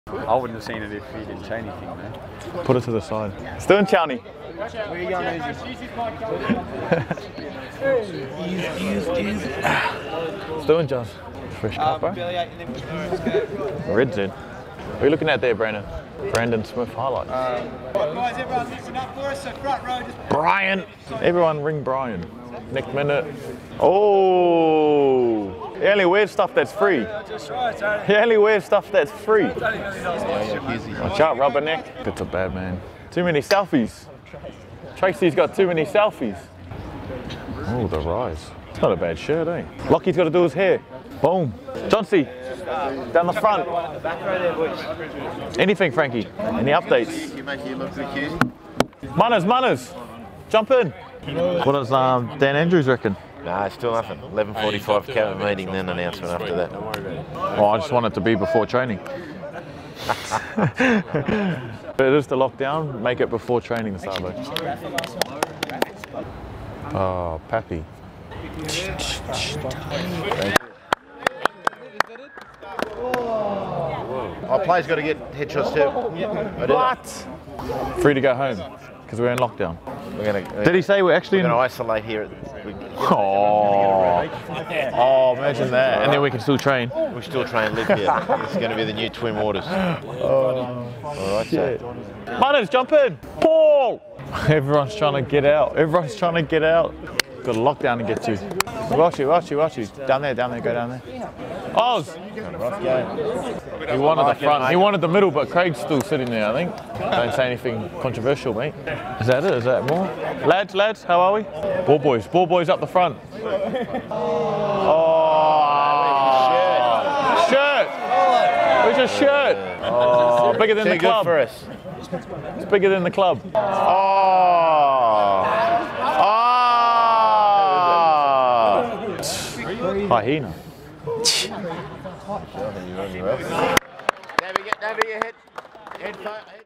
Jahrome Hughes Mic'd Up | Team Photo Day
We mic'd up Hughesy during our team photo day.